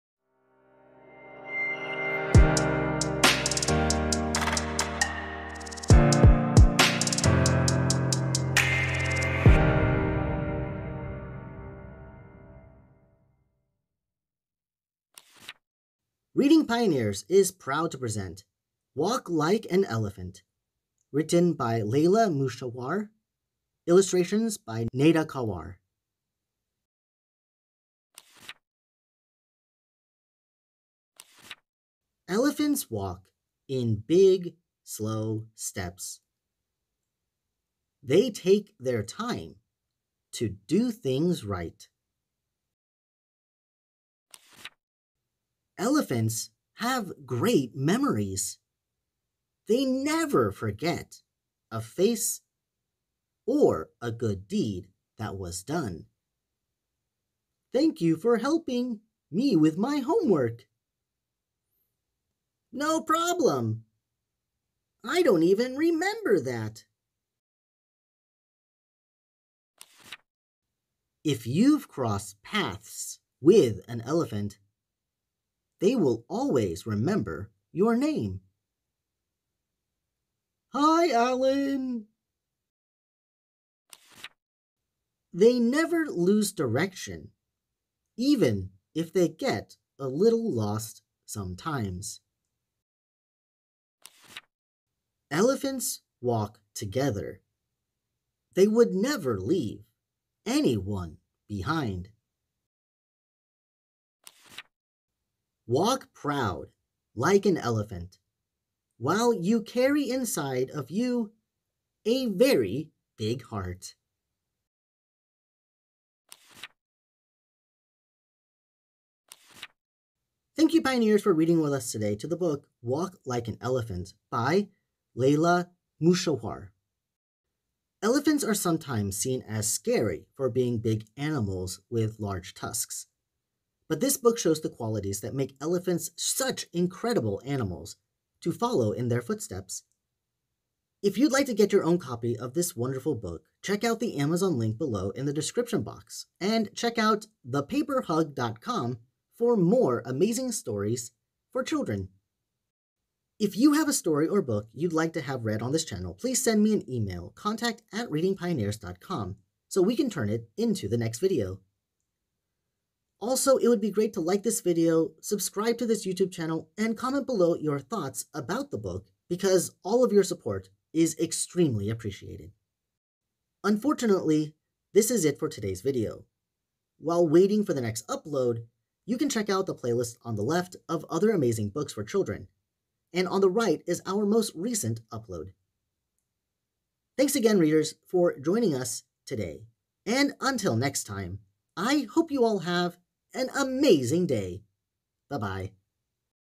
Walk Like an Elephant Read Aloud
Walk like an Elephant is about the large majestic animal that walks at its own pace and has the greatest memory of all the animals in all the jungles and the savannas. It is the first title in the Lessons from Animals Series. The Lessons from Animals picture book series allocates each title to a different animal, to highlight their positive behavior in a fun way so that the animals become role models for children.

To support Leila Mushahwar, and get your hands on your own copy of the book,